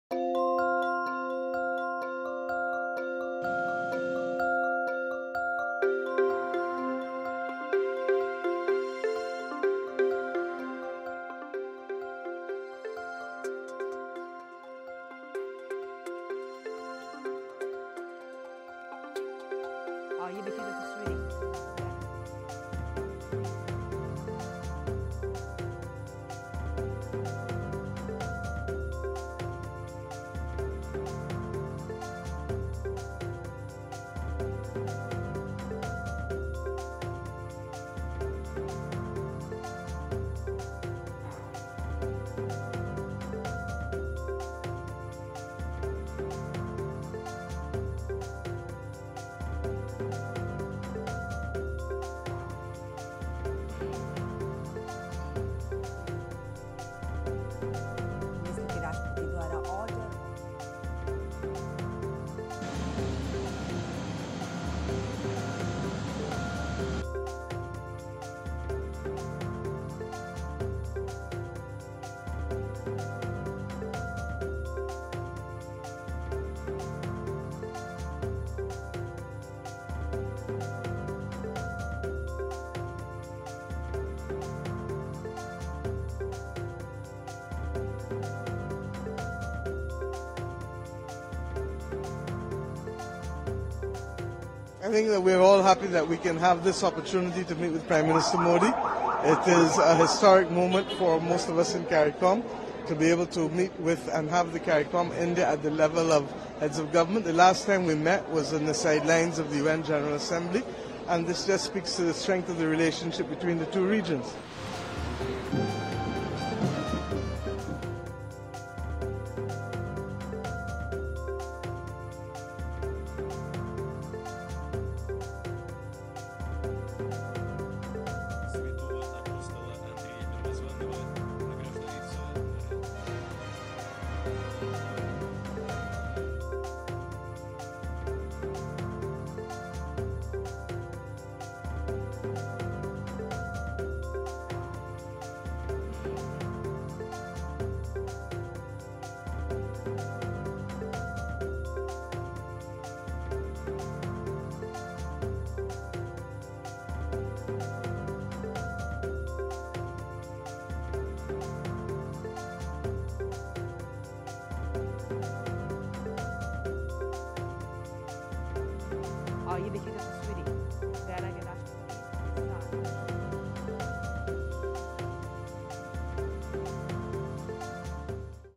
PM Modi In Guyana | PM Modi To Receive Prestigious Awards From Guyana And Barbados
Prime Minister Narendra Modi is set to receive two top honors during his state visit to Guyana and Barbados, further solidifying his growing list of international recognitions. Guyana will award him the highest national honor, “The Order of Excellence,” while Barbados will present the prestigious “Honorary Order of Freedom.” These accolades add to the 19 global honors PM Modi has received in the past decade, including Russia’s Order of St. Andrew the Apostle and the U.S. Legion of Merit. PM Modi's latest international recognition comes after his successful participation in the G20 summit in Brazil.